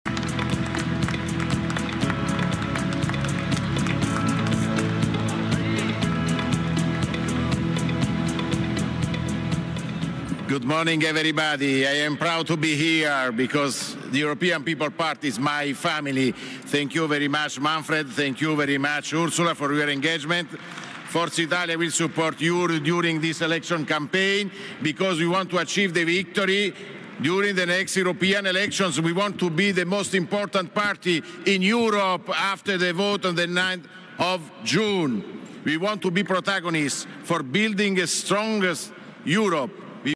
Tajani al congresso del Ppe: Dobbiamo essere partito più importante in Europa
(Agenzia Vista) "Sono orgoglioso di essere qui perché il Partito popolare europeo è la mia famiglia. Grazie mille vostro impegno. Sosterrò tutti voi durante questa campagna elettorale perché vogliamo ottenere la vittoria alle prossime elezioni europee e vogliamo essere il partito più importante in Europa dopo il voto della notte di giugno. Vogliamo essere protagonisti per costruire un’Europa più forte", le parole di Tajani al congresso del Ppe a Bucarest. / Epp